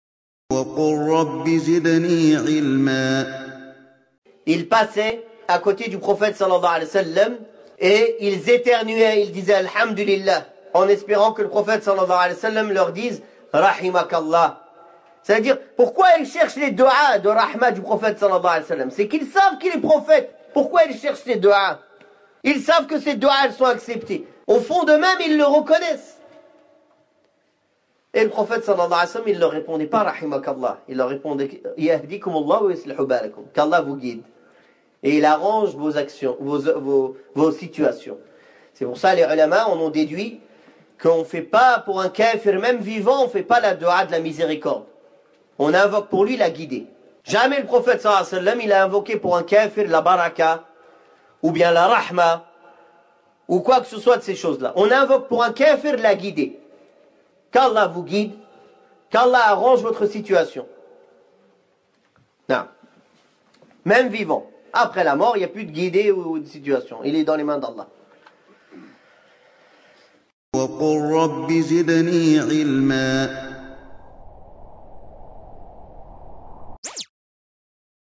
🔎📖 Les invocations autorisées pour les koufars (Abou Laïth 'Othmân Al Armany) - Dourous-Sounnah.com
Extrait_de_cours # 426: Les invocations autorisées pour les koufars
Tafsir 050 Al Baqarah (sourate La vache) versets 58-62
(2015)
Sourate 002